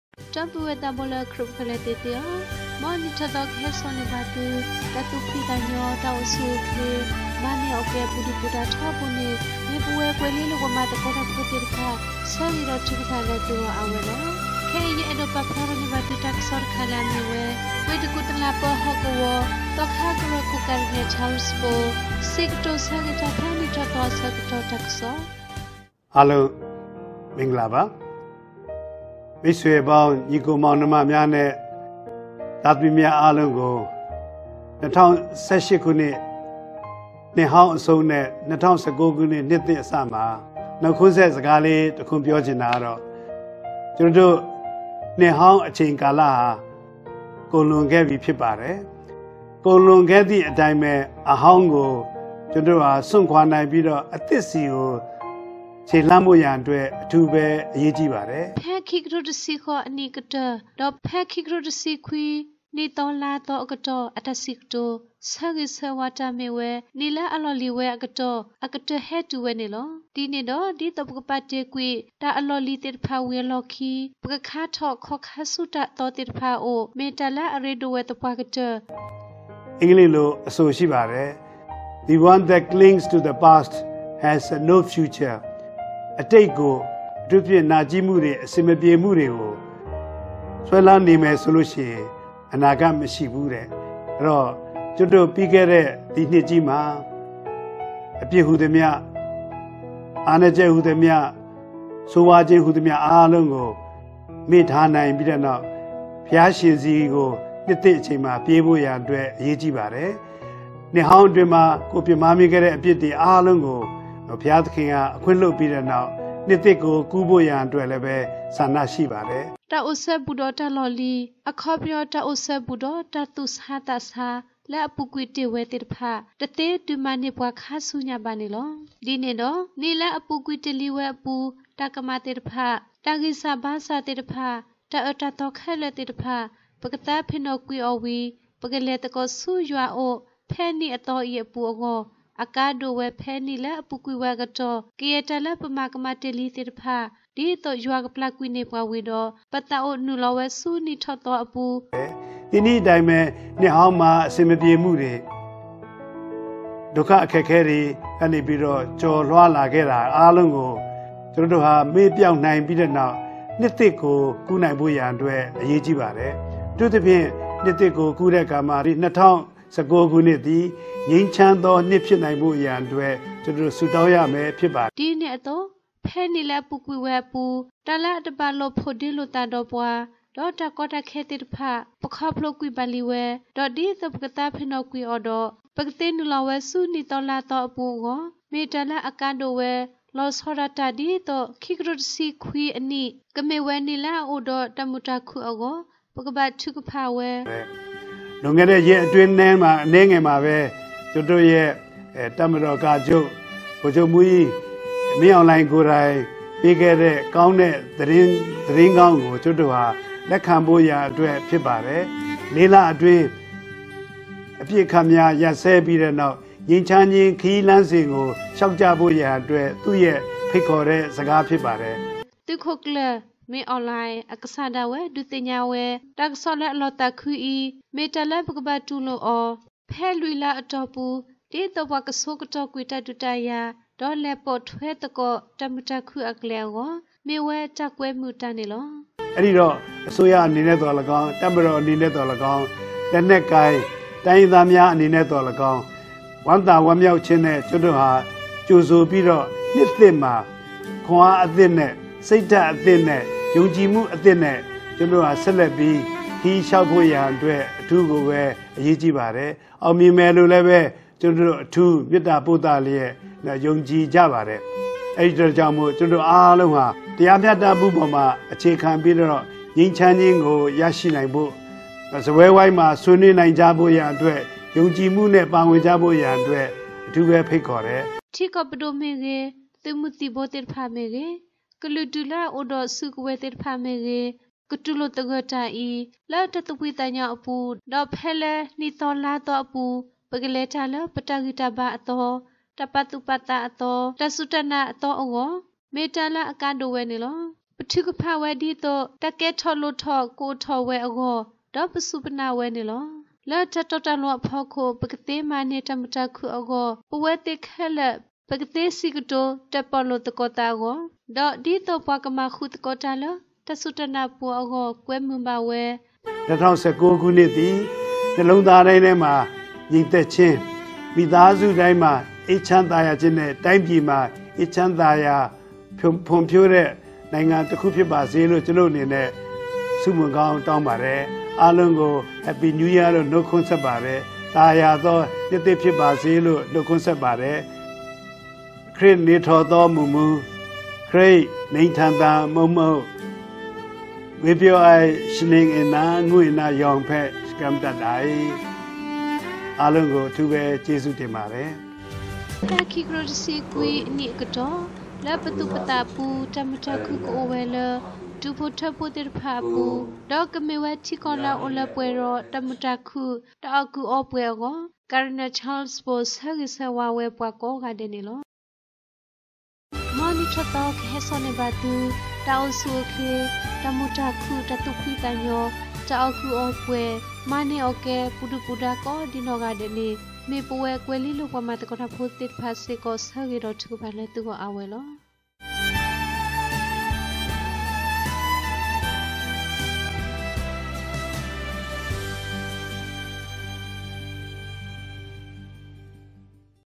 New Year Message by Cardinal Charles Bo.(29/12/2018) …
The New Year Message from Cardinal Charles Bo ; Now we are end of the year 2018,and 2019 year is coming, Let's receive God's forgiveness for all the sins and failures of past year 2018 and begin the New Year 2019 with love and goodness of the Lord.